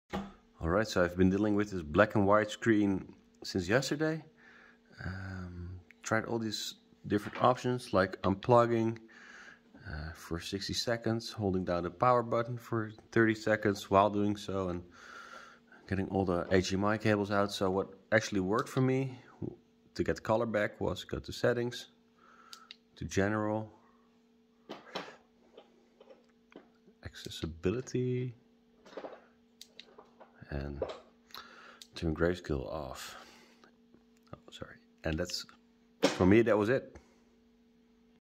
LG oled (g1, c1) black and white screen fix
LG G1 black and white screen back fix. Probably had something to do with our baby playing with the remote after all.